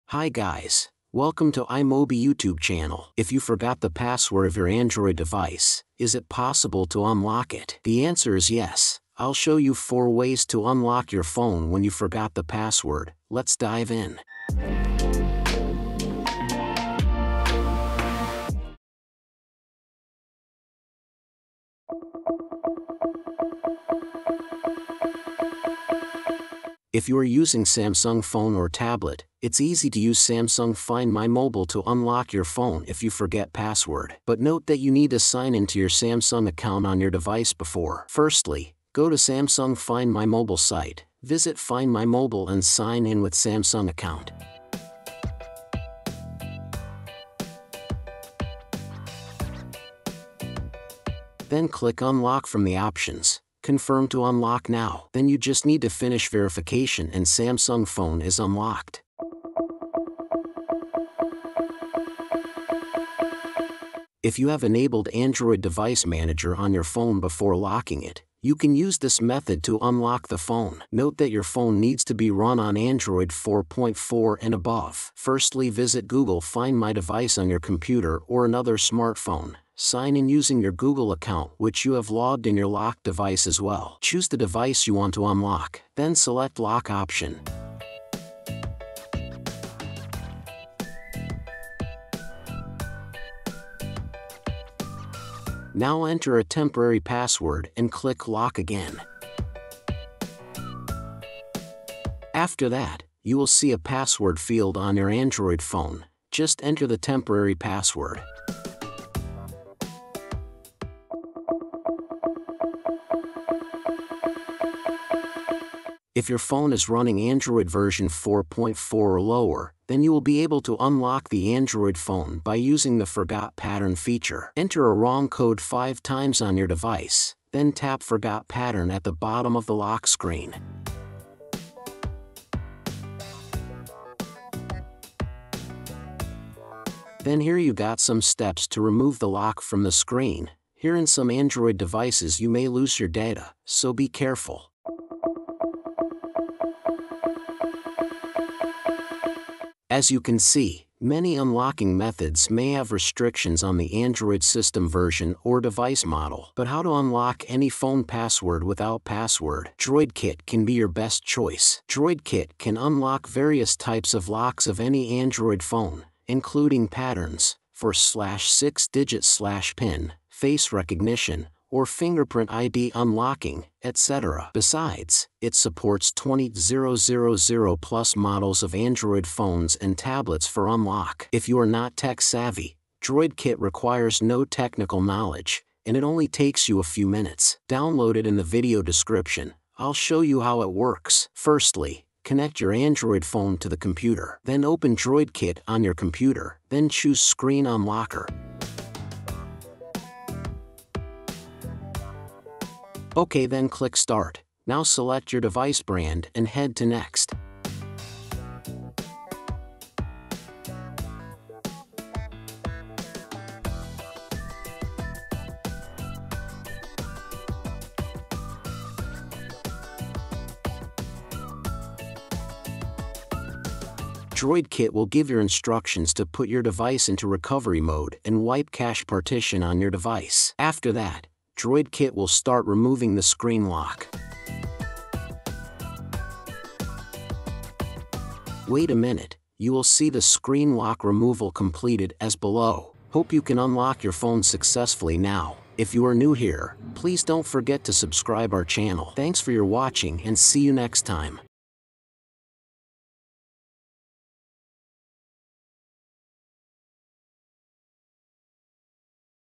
[2024 Updated] How to Unlock Your Phone When You Forgot the Password?
If you forgot the password of your Android device, is it possible to unlock it? The answer is yes! I'll show you 4 ways how to unlock your phone when you forgot the password.
00:00 Intro
00:24 Unlock Android Phone Password via Find My Mobile
01:03 Unlock Phone without Password via ADM Remotely
01:54 Unlock Phone without password via Google Account
02:26 Unlock Any locked Phone in DroidKit